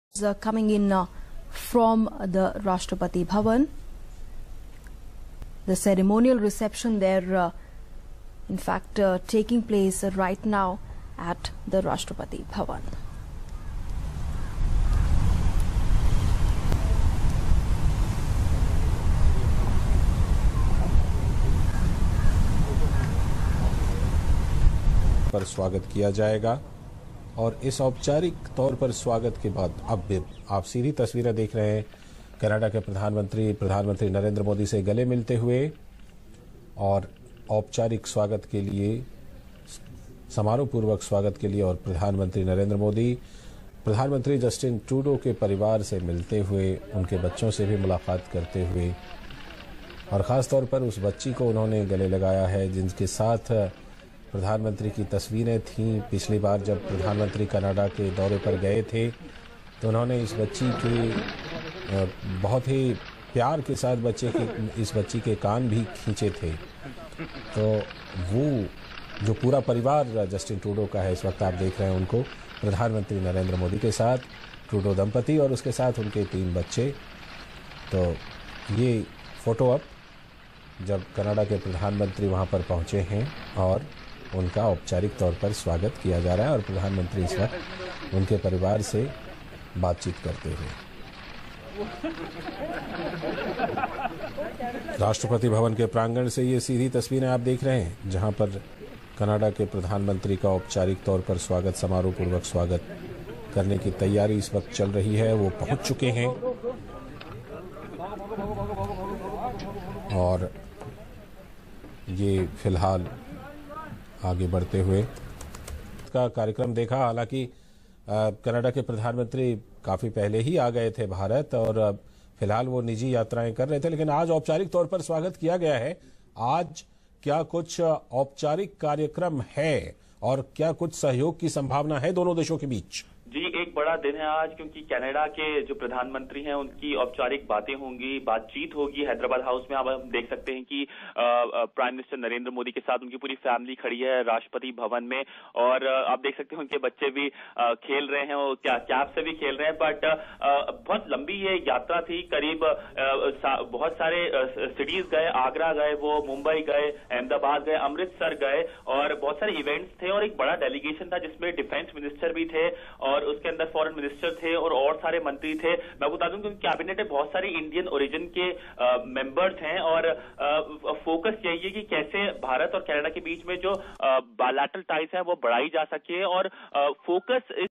PM Narendra Modi welcomes Canada PM Justin Trudeau at Rashtrapati bhawan
PM Narendra Modi welcomes Canada PM Justin Trudeau at Rashtrapati bhawan India New Delhi